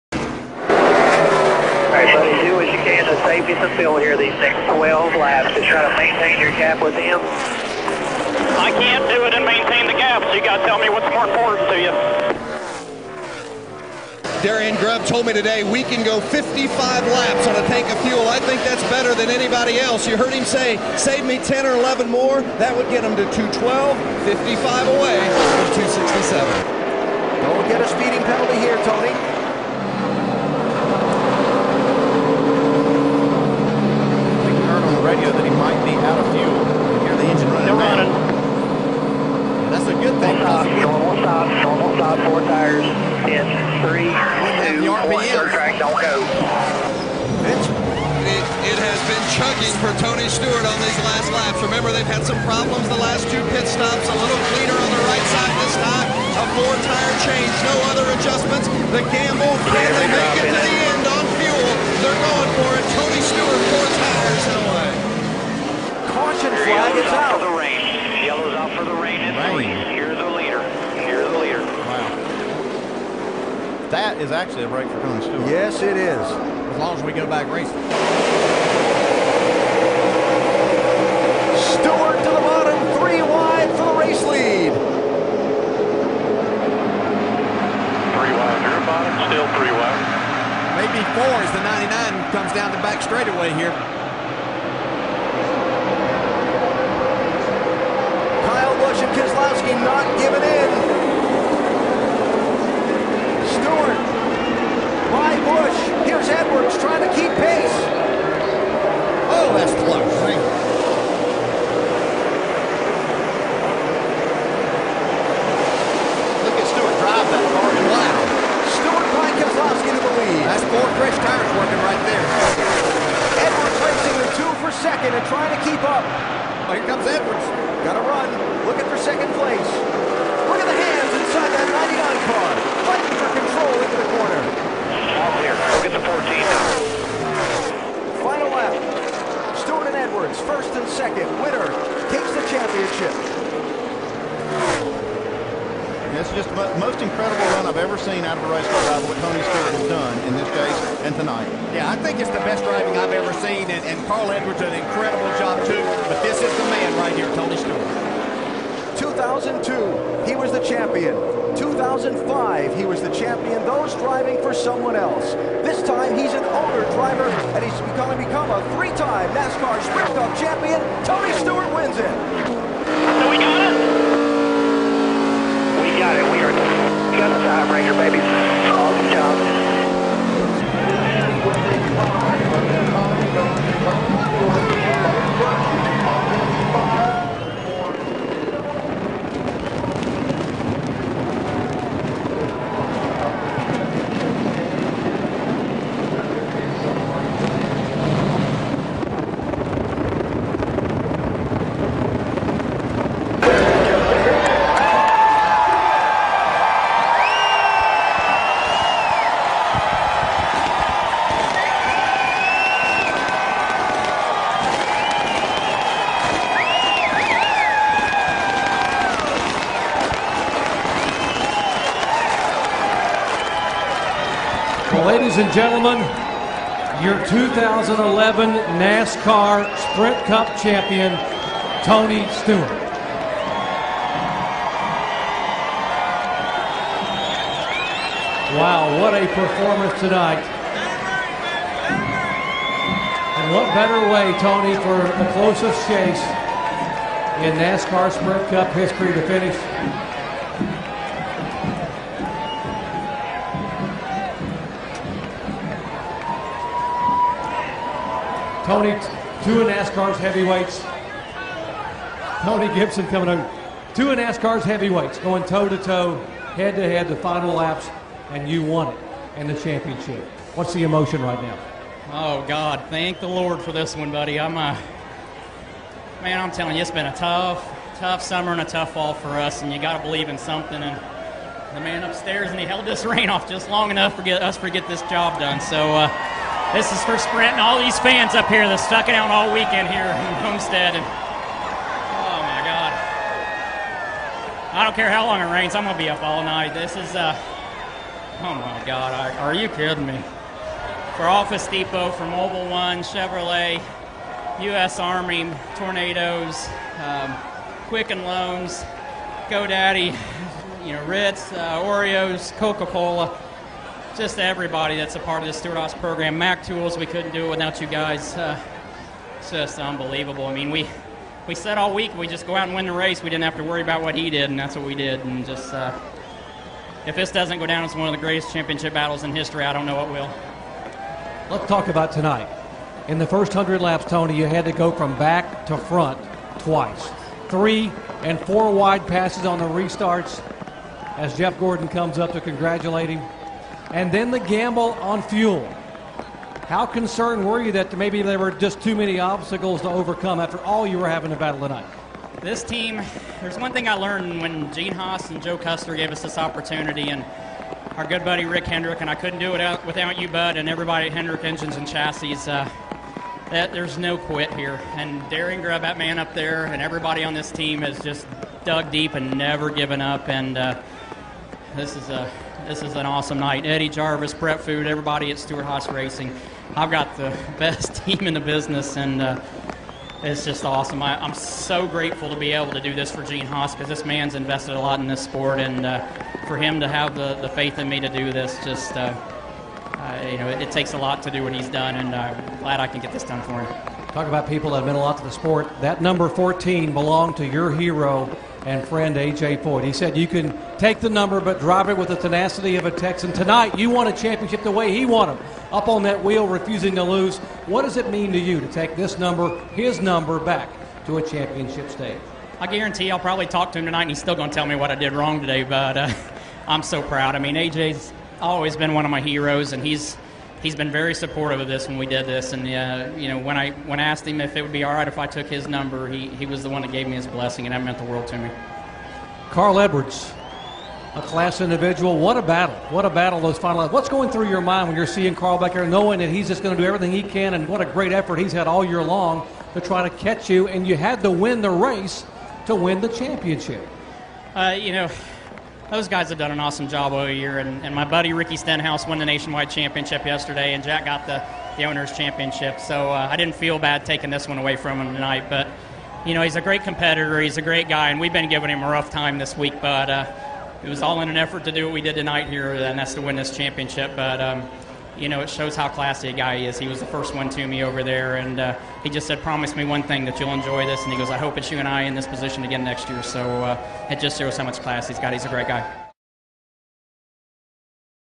Tony Stewart Wins the 2011 NASCAR Sprint Cup Championship/Ford 400!
After thinking it would be a struggle for Tony to get into the Chase, he then dominated going in by winning 5 of the 10 races, and beating Carl Edwards in NASCAR's first ever tiebreaker! Awesome job Tony to win the race and the Championship!